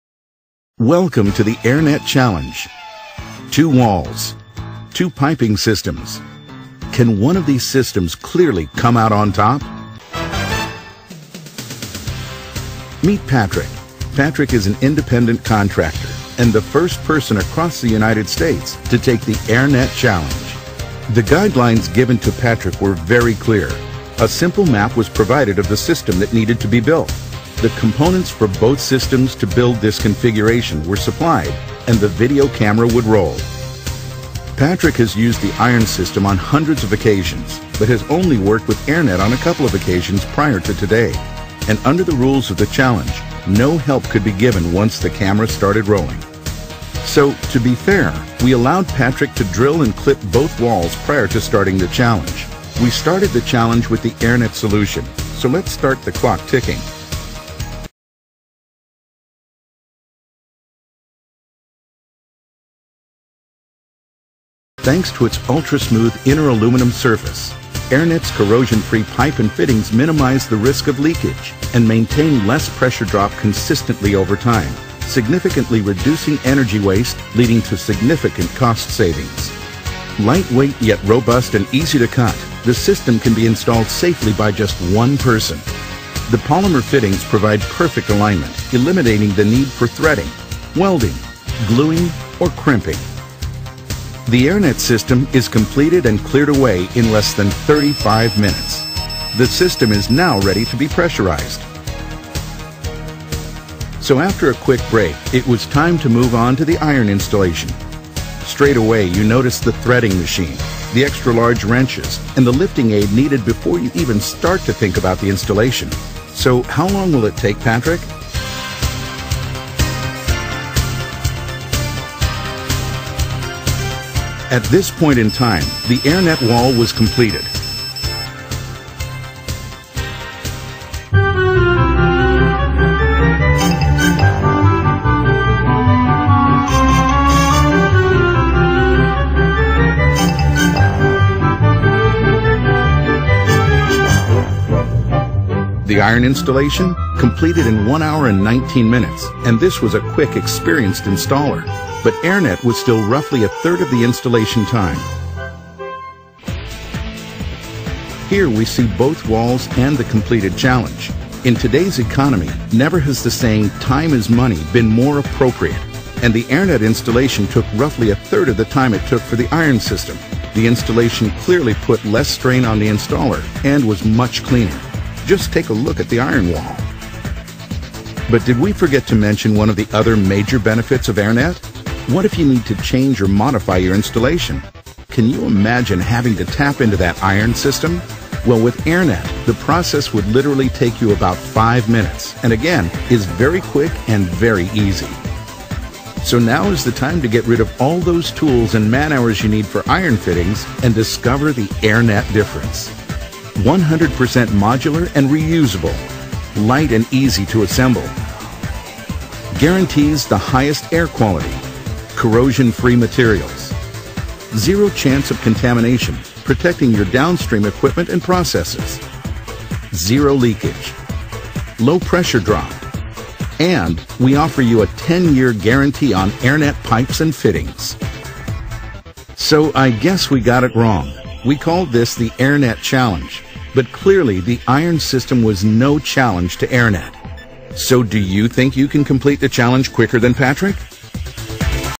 AIRnet vs Galva leidingen
Vergelijking in installatietijd tussen het AIRnet perslucht leiding systeem en het klassieke Galva leiding systeem. Heeft u vragen i.v.m. perslucht producten of installaties?